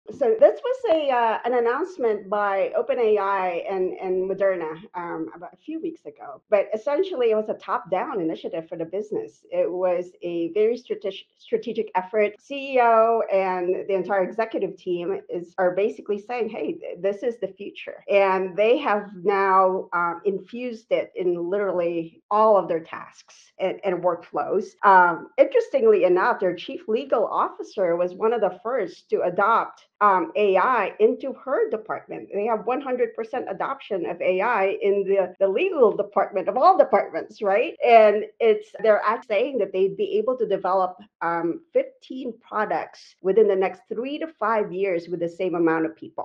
Strategic AI Adoption
How OpenAI and Moderna are Leading the Charge with AI Adoption

In a recent podcast with Jon Russo and Megan Heuer, Liza Adams, AI expert and Fractional CMO discussed a groundbreaking initiative announced by OpenAI and Moderna.

This strategic move aims to integrate AI into all facets of their operations.
This innovative approach is expected to enable the development with their current workforce.

Here's an excerpt:

"So this was an announcement by Open AI and Moderna about a few weeks ago
But essentially it was a top-down initiative for the business
It was a very strategic effort

CEO and the entire executive team are basically saying “Hey this is the future”

And they have now infused it in literally all of their tasks and and workflows

Interestingly enough, their chief legal officer was one of the first to adopt AI into her department

They have 100% adoption of AI in the legal department of all departments, right?

And it's their act saying that they'd be able to develop 15 products within the next three to five years with the same amount of people"

👏 Follow B2B Fusion on Go To Market best practice strategies.
🤙🏽 Struggling with faster, more efficient growth? Let's talk.